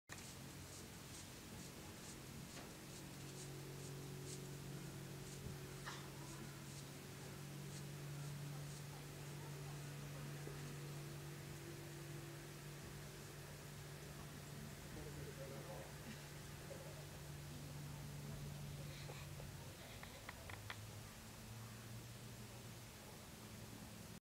Storchkücken mit mutigen Flügelschlägen im Nest
Storchbaby macht schon fortschritte im Flügelschlagen